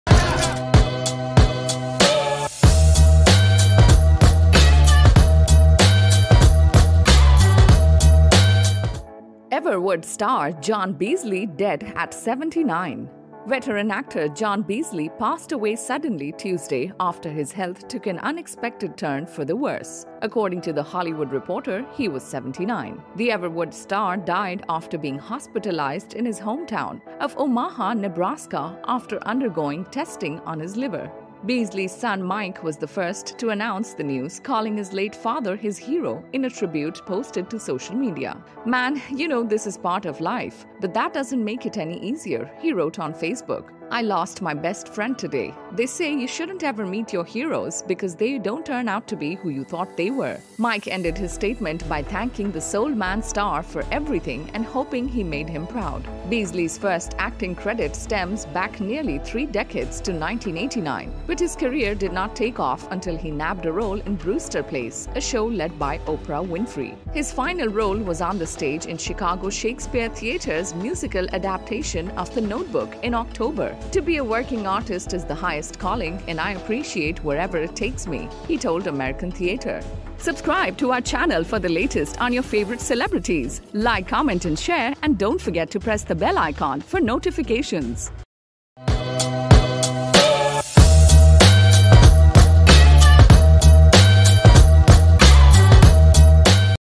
Star John Beasley dead at 79
Veteran actor John Beasley passed away suddenly Tuesday after his health “took an unexpected turn for the worse,” according to The Hollywood Reporter. He was 79.